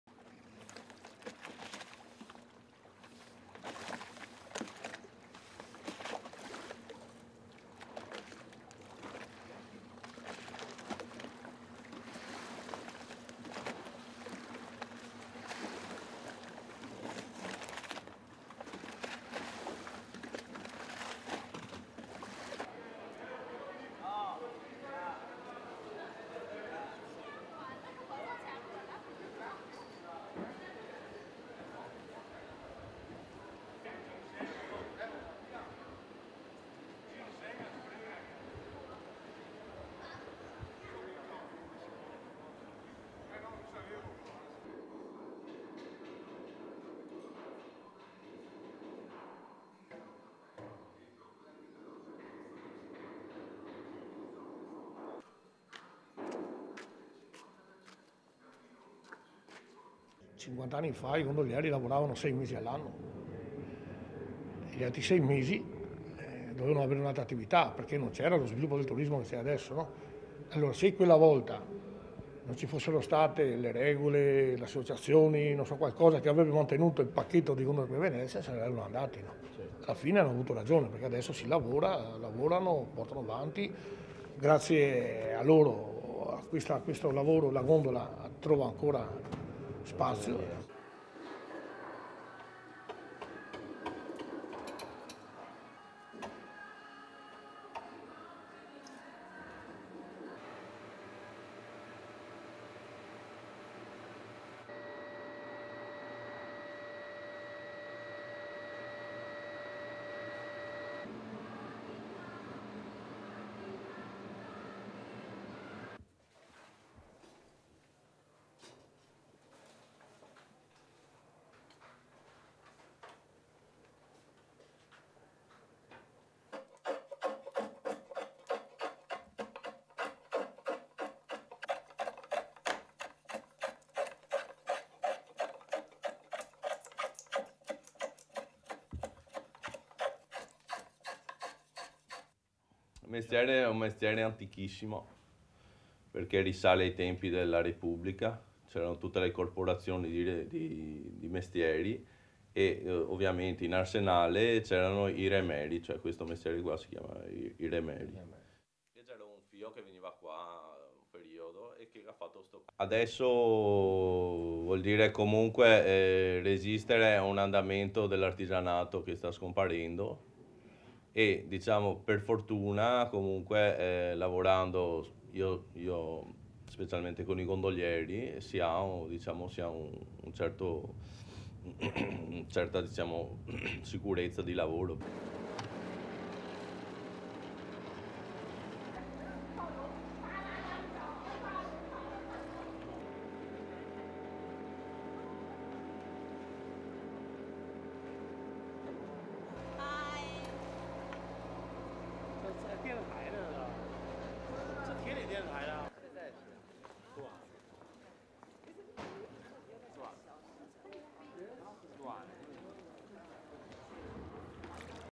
The ancient gondola trade resisting the recession
Venice, February 13, 2009
1. Various of gondolas in harbour at sunrise swaying on the water
2. Mid shot of gondolier helping tourists onto a gondola
3. Tilt down of gondolier rowing his gondola
4. Various of Gianfranco Vianello, owner of gondola shipyard, working in shipyard
5. SOUNDBITE: (Italian) Gianfranco Vianello, Gondola maker
"Fifty years ago gondoliers worked six months a year only, the other six months they had to find another activity because there was not the kind of tourism we see today. If at that time there hadn't been rules, associations to protect this category in Venice, gondolas would now be gone. At the end they were right to do so since now we all work and continue to work and thanks to such measures the gondolas still exist today."
6. Various of workers engraving wood in gondola shipyard
7. Various of patterns engraved onto wood in gondola shipyard
8. Mid shot of tip of gondola in gondola shipyard
9. Various of exterior of workshop with man working inside
10. Various of Paolo Brandolisio, artisan who makes oars and oarlocks, working on wooden oarlock
11. Mid shot of black and white photograph of Paolo Brandolisio as a young boy with previous owner of workshop Giuseppe Carli
12. SOUNDBITE: (Italian) Paolo Brandolisio, Venetian artisan
"This is a very ancient job which dates back to the Venetian Republic. There were all the guilds and in the arsenal area there were 'remeri' oarsmakers. This is how this job became called 'remeri', and it is then that the oarsmakers started this job."
13. Close of wooden oarlocks
14. SOUNDBITE: (Italian) Paolo Brandolisio, Venetian artisan
"Now doing this job means anyway resisting since it's a craftsmanship which is disappearing, and, let's say luckily, for me working with the gondoliers means a certain security of work."
15. Wide shot of two gondolas with tourists rowing up a Venice canal
16. Close of gondolier's face
17. Close of oar moving in the water
18. Various of tourists waving as gondola sails under Venice bridge
LEAD IN :
Venice is one of the world's most popular tourist destinations.
The Italian city's canals make a romantic journey by gondola as 'must do' on any visitors' list.
The historic craft of gondola making is thriving thanks to the enduring popularity of the boats.
STORYLINE:
As dawn breaks over Venice's canals, the city's famous gondolas bob up and down in the ebb and flow of the tide.
For now at least all is quiet.
But soon the tourists begin to arrive and the great Venice gondola traffic jam begins.
Gondola makers are thriving now as tourists flock to Italy's most unique city.
More than one (m)illion visitors attended the 2009 Carnival - the highest number in the last seven years.
But the future of gondola artisans was not always so assured.
In XVI century gondola was the most common boat used in Venice, every family owned one, according to the Venetian Institute for the Conservation of the Gondola.
The institute estimates that in 1580 there were more than 10,000 gondolas, whereas nowadays there are around 700.
Nowadays there are 4 'squeri', gondolas boatyards, remaining in Venice.
Each year they produce between10 to 15 new gondolas to replace old ones, each vessel has a lifespan of twenty years.
Manufacturing a gondola requires more than 400 hours of work and costs between 25,000 to 30,000 Euro. (30,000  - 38,000 US dollars)
Venetian Gianfranco Vianello worked as a gondolier for 35 years and now owns his own gondola shipyard, or as Venetians call it, his own 'squero'.
He has not lost passion for this ancient means of transport.